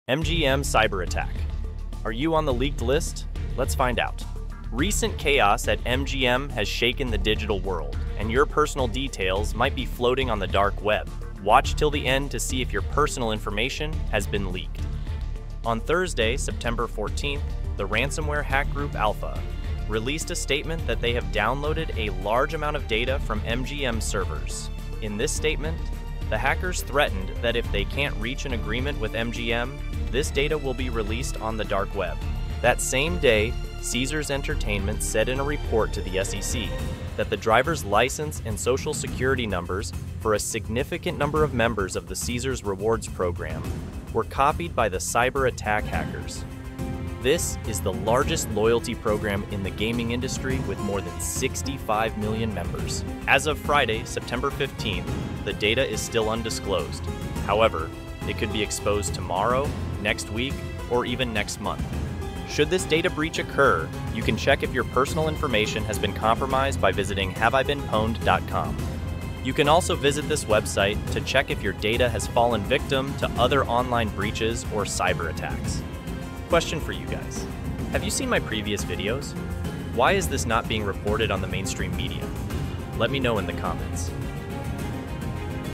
MGM Cyber Attack: Are YOU on The Dark Web?
MGM Cyber Attack: Are YOU on the Leaked List? Let’s find out.

Recent chaos at MGM has shaken the digital world, and your personal details might be floating on the dark web. Watch till the end to see if your personal information has been leaked.

On Thursday, September 14th, the ransomware hack group ALPHV, released a statement that they have downloaded a large amount of data from MGMs servers. In this statement, the hackers threatened that if they can’t reach an agreement with MGM, this data will be released on the dark web.

That same day, Caesars Entertainment said in a report to the SEC, that the driver’s license and social security numbers “for a significant number of members” of the Caesars Rewards program, were copied by the cyber attack hackers. This is the largest loyalty program in the gaming industry with more than 65 million members.

As of Friday, September 15th, the data is still undisclosed. However, it could be exposed tomorrow, next week, or even next month.

Should this data breach occur, you can check if your personal information has been compromised by visiting this website.

You can also visit this website to check if your data has fallen victim to other online breaches or cyber attacks.

Question for you guys. Have you seen my previous videos? Why is this not being reported on the mainstream media?